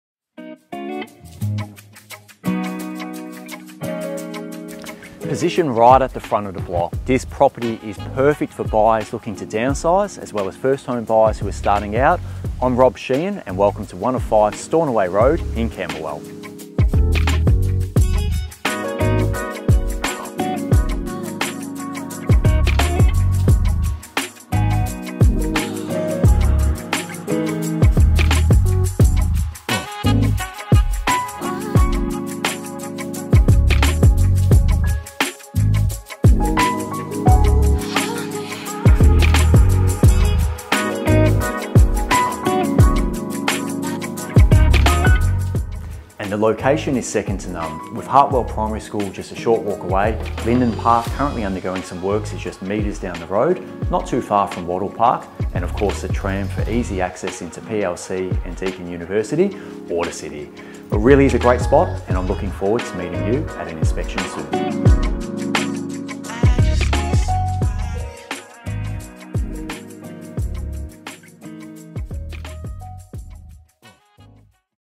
Fletchers - 1/5 Stornoway Road, Camberwell - Rob Sheahan
Downsize effortlessly, invest with supreme confidence or take that first energising step into home ownership when you secure this peaceful three bedroom home.

Established at the front of an attractively leafy block and spread across one comfortable level, front garden views provide a lovely backdrop to the generous lounge and dining room. With honeyed polished boards underfoot, this space extends into the neatly presented kitchen, looking out to a serene courtyard.

In a cherished locale just two doors away from expansive Lynden Park, walk to Hartwell Primary School and the train station. Near Burwood Village cafes and Leo’s Supermarket, jump on the tram for a quick trip to Presbyterian Ladies’ College, Deakin University and Camberwell Junction.

Visit the Fletchers website for more information.